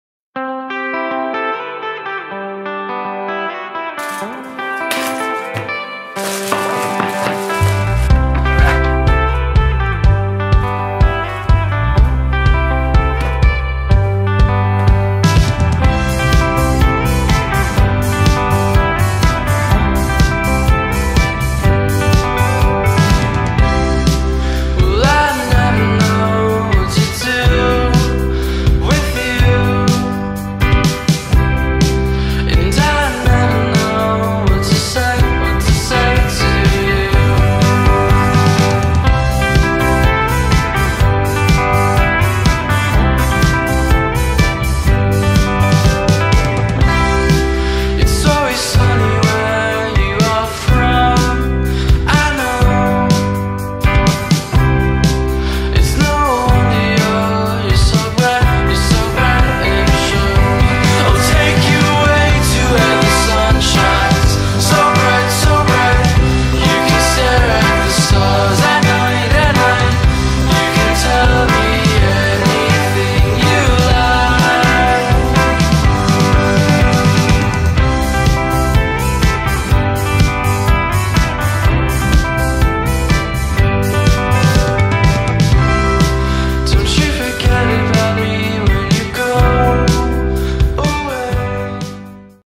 Skateboard Setup
Just fresh deck and a set of wheels to open up the spring season! A nice birthday gift and a cruise down memory lane.